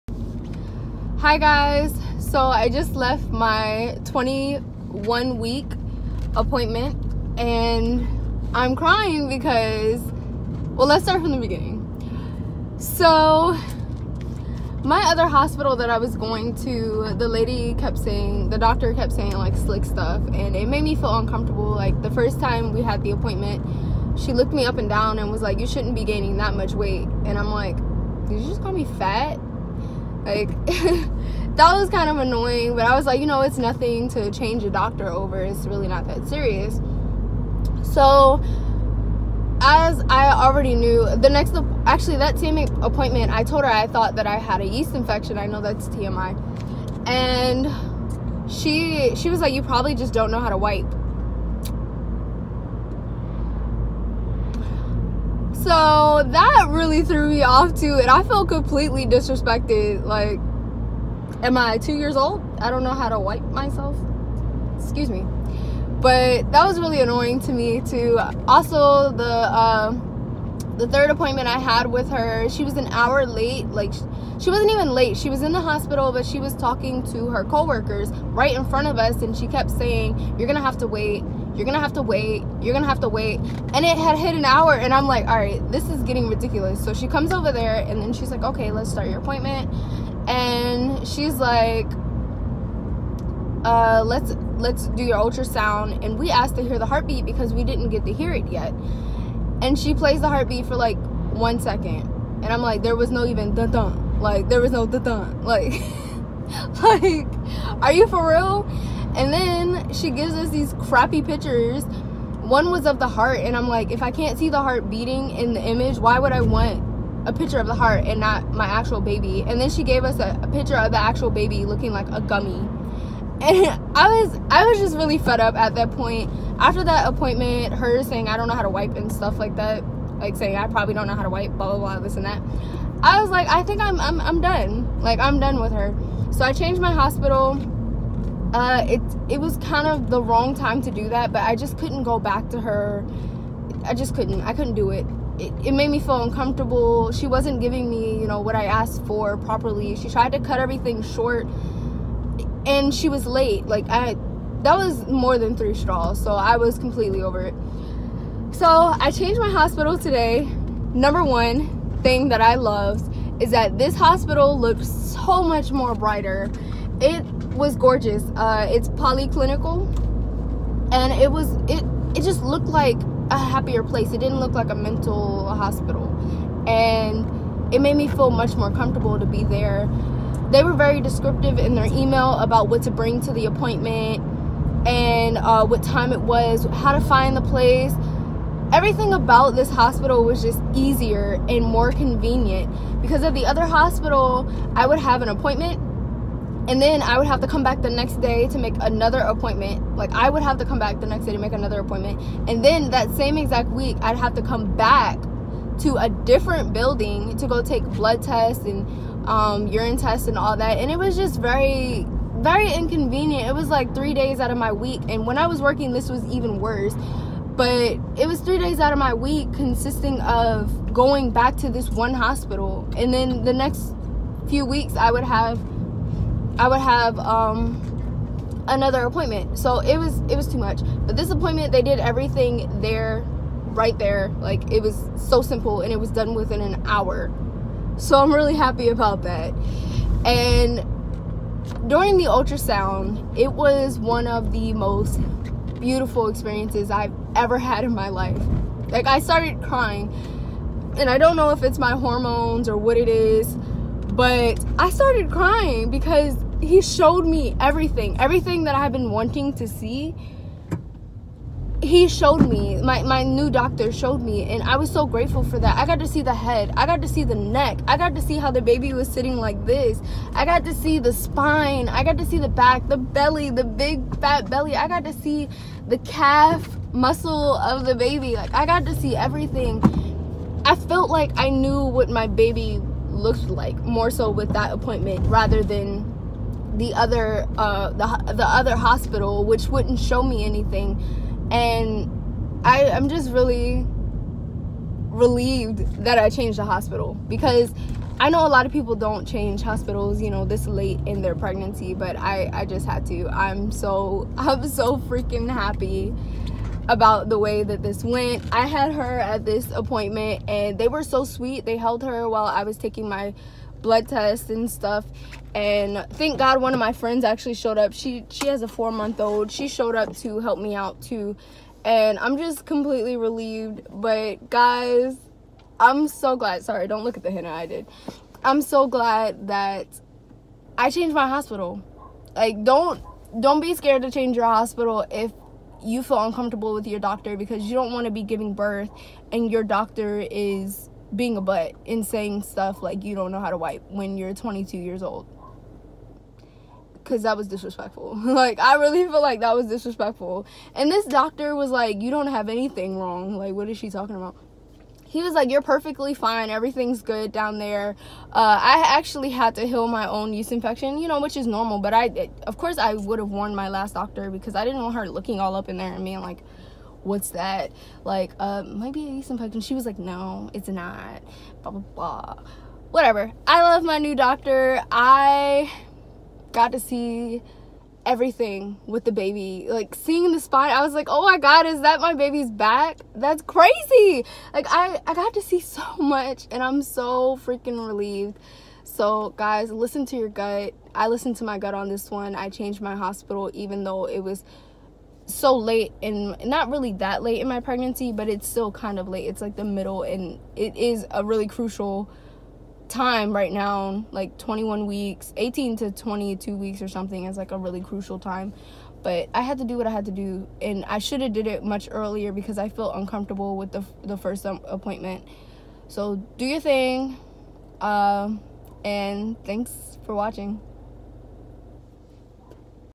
Changed My Birth Hospital At 5 Months Pregnant | TMI & Drama
Sometimes you have to listen to your gut and do what you have to do.
I definitely recommend everyone listening to their intuition when it comes to discomfort and trust. I unfortunately didn't feel comfortable with the orginal hospital I went to. Due to dirty looks, rude comments and constantly being treated like I didn't deserve what other people at that hospital got.

Ladies don’t be embarrassed about yeast infections. You can become more prone to them during pregnancy due to your ph balance and body changes.

My doctor informed me that it wasn’t actually a yeast infection and it was just extra discharge due to being pregnant. Definitely be aware of that. I didn’t have itching and no smell, it was just different for me. SO CHECK WITH YOUR DOCTOR.

Also! Water will help with your ph balance!